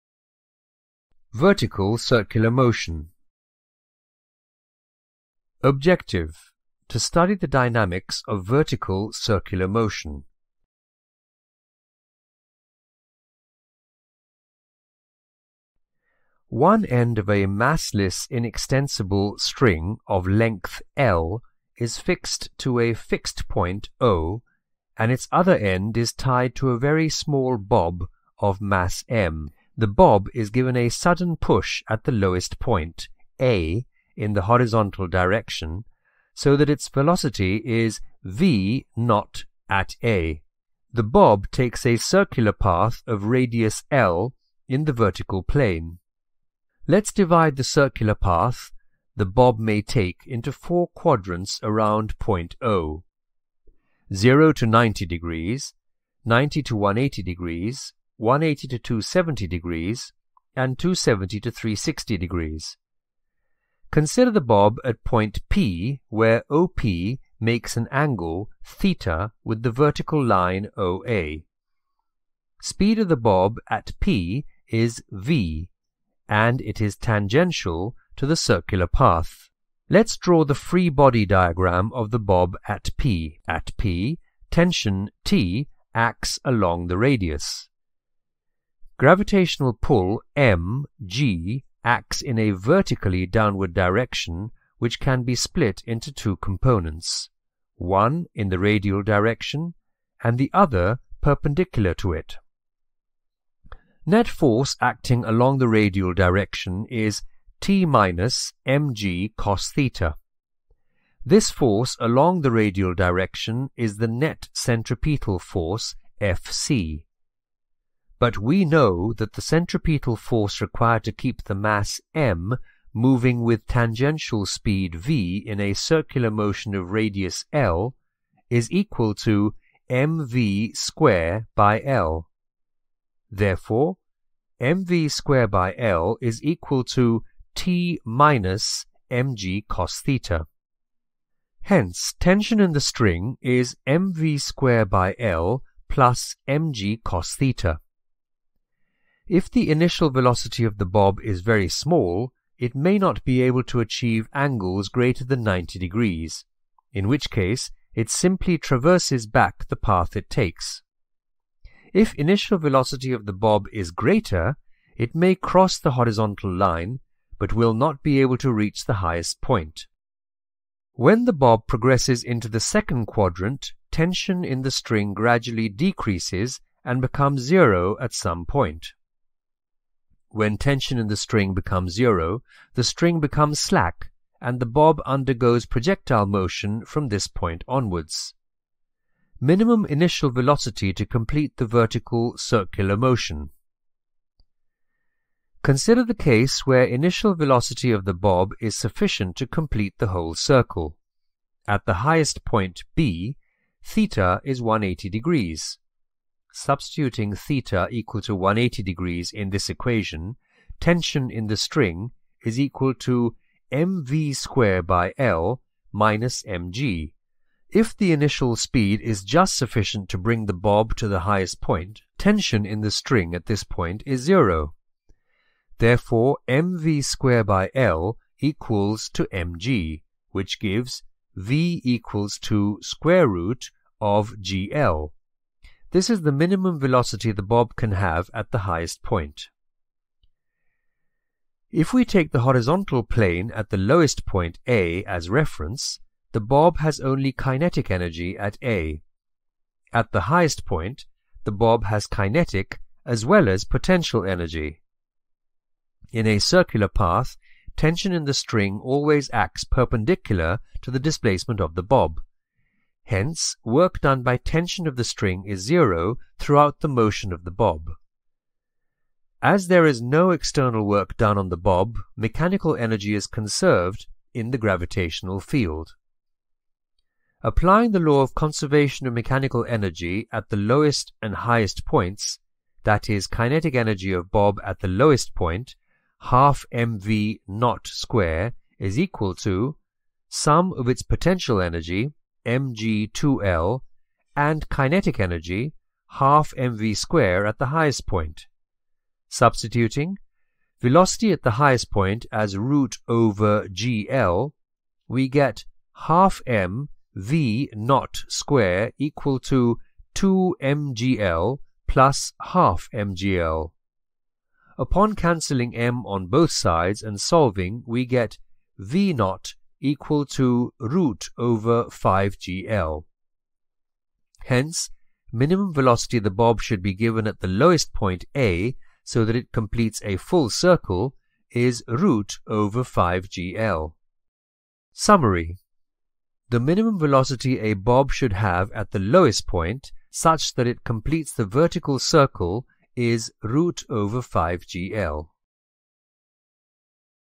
Practically's Concepts - Vertical Circular Motion
Concepts of the video:

0:00 - 0:10 : Introduction of Vertical Circular Motion
0:11 - 0:28 : Understanding the dynamics of Vertical Circular Motion with an experimental setup
0:29 - 1:03 : Considering a bob take a circular path that is divided into four quadrants
1:04 - 1:19 : Explanation of the angle, speed, tangential of the bob at point P
1:20 - 1:42 : Explanation of the tension, gravitational pull of the bob at P with free body diagram
1:43 - 2:28 : Explanation of the tension of the string of the bob
2:29 - 2:50 : Explanation of the tension if the initial velocity of the bob is very small or greater
2:51 - 3:08 : Explanation of the tension when it decreases and becomes zero at some point
3:09 - 3:13 : Explaining minimum initial velocity to complete the vertical circular motion
3:14 - 3:59 : Explaining the minimum velocity the bob can have at the highest point.
4:00 - 4:35 : Explanation of the kinetic and potential energy of the bob in circular path
4:36 - 5:34 : Explaining the equation of minimum velocity applying the law of conservation of mechanical energy at the lowest and highest points
5:35 - 5:47 : SUMMARY

Don't just learn, learn Practically!